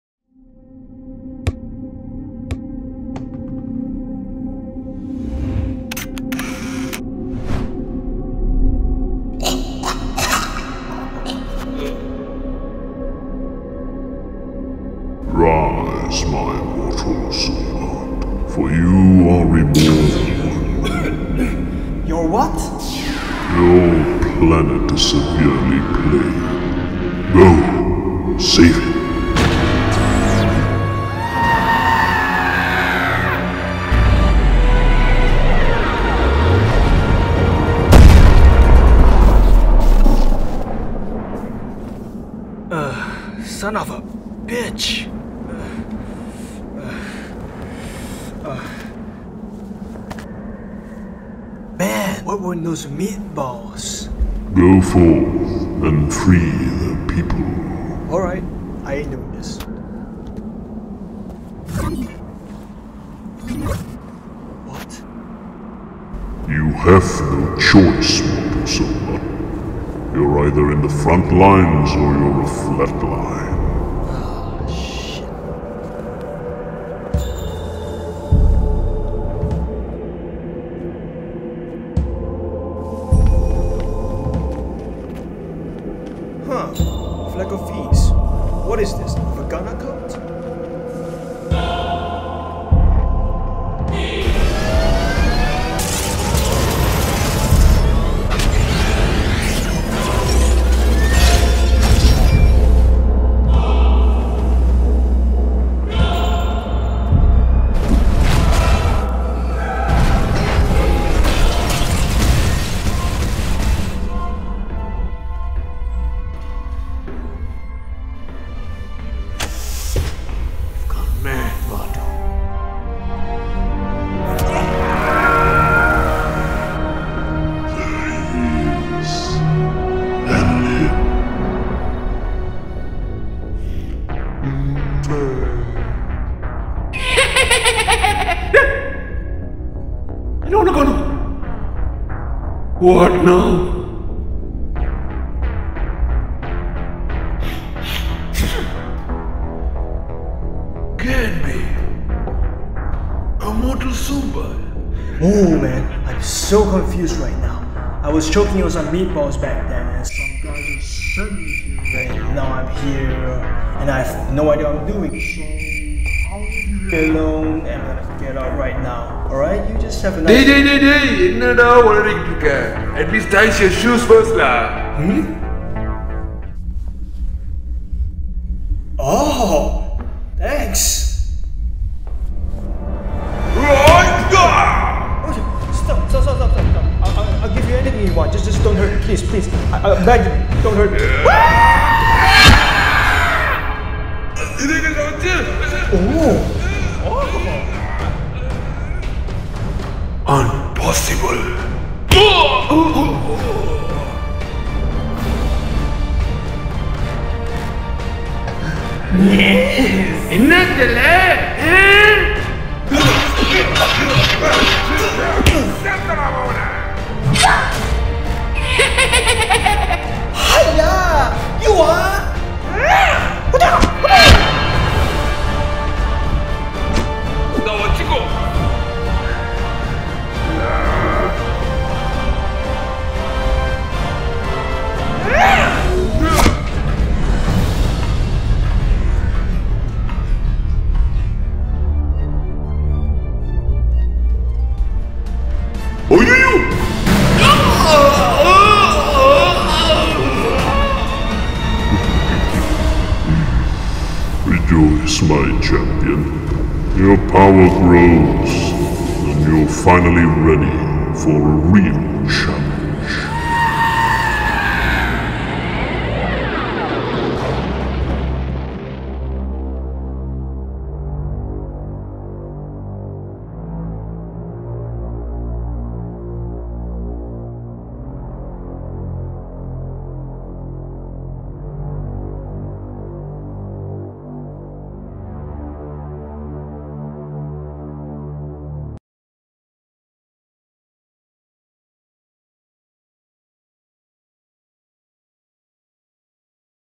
Mortal Sumbat | Episode 1: Chosen One
A donut shaped planet is in some kind of danger. A man who died chocking on meatballs has been reborn as a hero. An Unreluctant Hero. The best regret you will ever have.

Audios used does not belong to us. This magnificent garbage is the works of students and is not intended for profit.